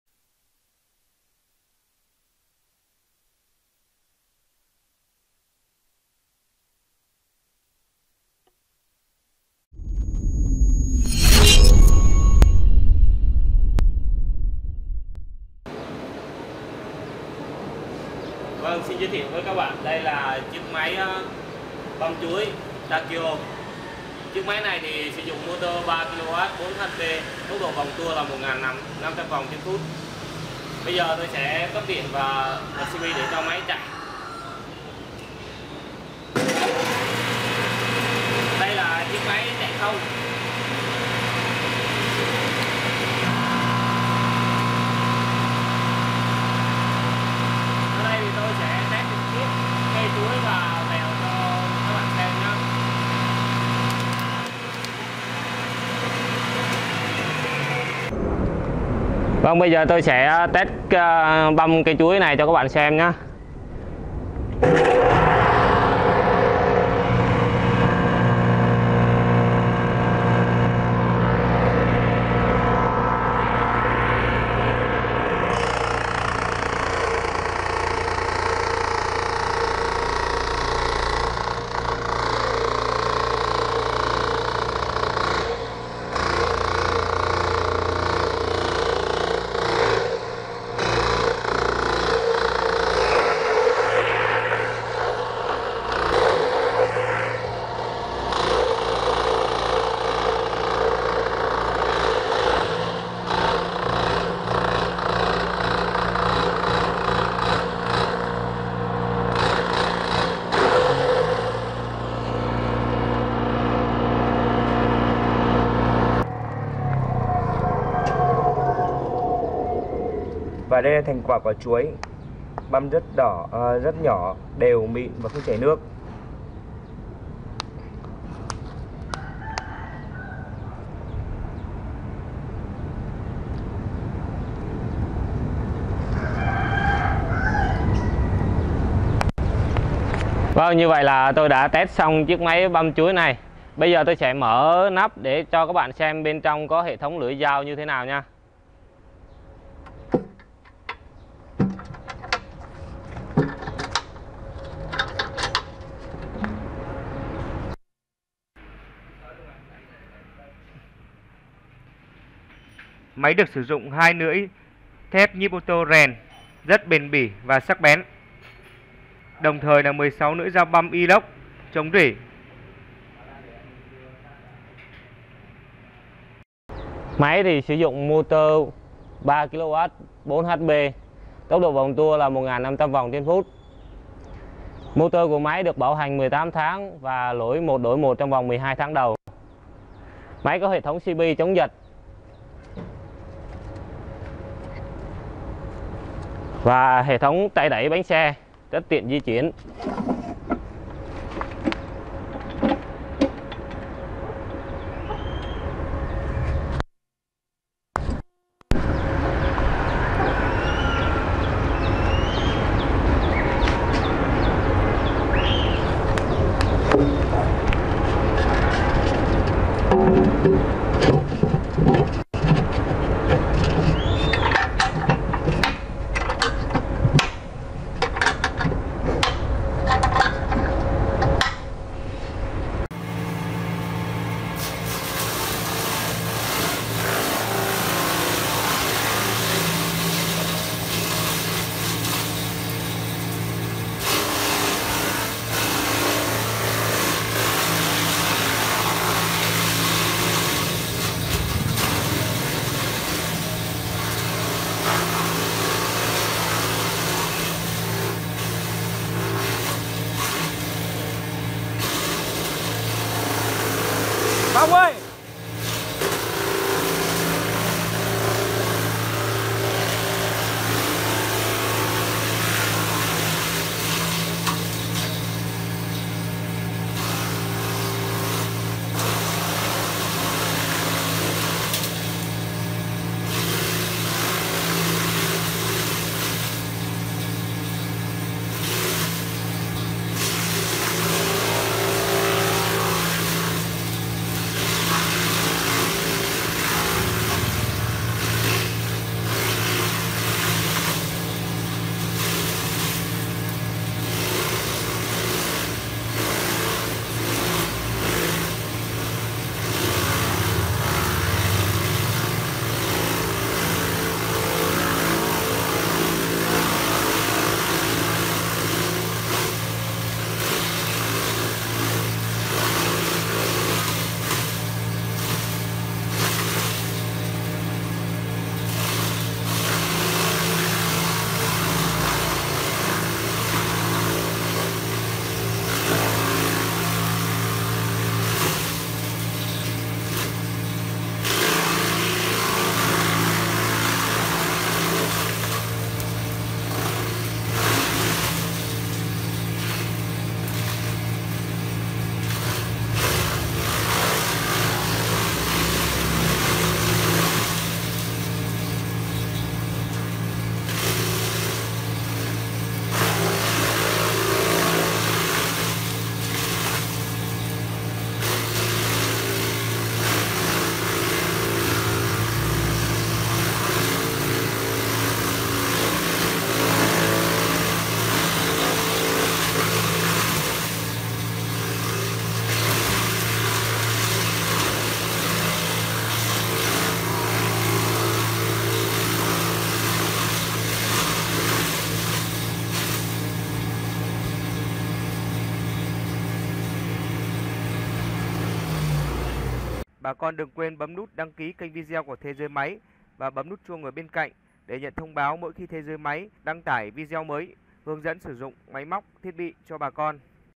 Máy băm chuối takyo mới nhất ở Bạc liêu, Bán máy băm chuối đa năng tại bạc liêu
✅ MÁY BĂM CHUỐI TAKYO TK3000 GIÁ: 6,000,000 VNĐ✅ Bảo hành 18 tháng
✅16 lưỡi băm inox✅2 lưỡi thái thép rèn nhíp oto✅khung chân sắt v chắc chắn✅tay đẩy bánh xe✅bánh xe di chuyển cao su đặc✅thùng bằng thép mạ kẽm chống han gỉ✅trục thép công nghiệp cmc✅bạc đạn chịu nhiệt của nhật✅cp bảo vể motor✅motor dây đồng 100%✅
✅ BĂM CÂY CHUỐI✅Băm cỏ voi✅Băm lục bình✅Băm rau✅Băm thân cây bắp
✅- ƯU ĐIỂM CỦA MÁY BĂM CHUỐI HỖN HỢP TAKYO TK3000:
- Máy được thiết kế đơn giản gọn nhẹ, dễ dàng sử dụng phù hợp với nhà nông, đặc biệt máy băm đa năng được thiết kế bộ dao bao gồm 1 dao to 2 lưỡi , và 14 dao inox băm nhỏ chống gỉ.
- Máy được cải tiến thêm nắp cửa xả nguyên liệu để bà con có thể điều chỉnh nguyên liệu xả ra ngoài gần hoặc xa theo ý muốn của bà con
- Hệ thống băm được thiết kế phần nạp nguyên liệu rộng hình phễu rất tiện cho các loại rau cỏ cồng kềnh, đặc biệt là cây chuối
- Máy sử dụng động cơ 3 kw điện 1 pha và 3 pha, thích hợp với các mô hình trang trại có quy mô rộng, vật nuôi đa dạng.
✅--- THÔNG SỐ KỸ THUẬT CỦA MÁY BĂM CHUỐI HỖN HỢP TAKYO TK3000:
-Trọng lượng máy
-Nguyên bộ   63 ( kg)
-Không động cơ   42 ( kg)
-Hai lưỡi giao thái thép rèn công nghiệp
-14 Lưỡi giao inox băm
- máy có tay cầm và bánh xe di chuyển
✅--- LƯU Ý KHI SỬ DỤNG MÁY BĂM CHUỐI HỖN HỢP TAKYO TK3000
 - Lắp đặt máy ở nơi bằng phẳng
- Khởi động máy sau đó đưa nguyên liệu cần băm vào cửa nạp nguyên liệu, hệ thống dao sẽ làm việc và đưa ra cửa xả nguyên liệu.
- Vận hành xong chờ máy gạt hết nguyên liệu ra khỏi máy rồi tắt máy.
- Vệ sinh sạch máy sau khi sử dụng xong giúp máy có tuổi thọ cao hơn.